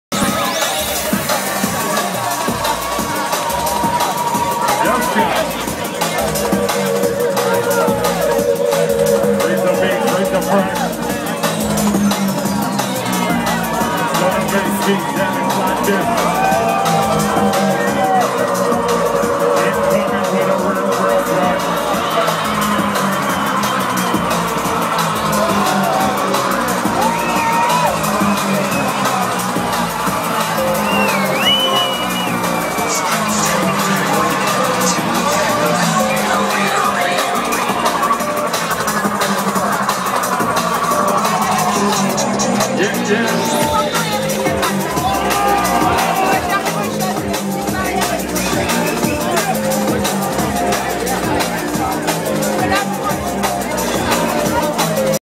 FD @ Sun And Bass 2012
FD for Critical Music Night @ Ambra Night (Outdoor Area), Sat 1st Sept 2012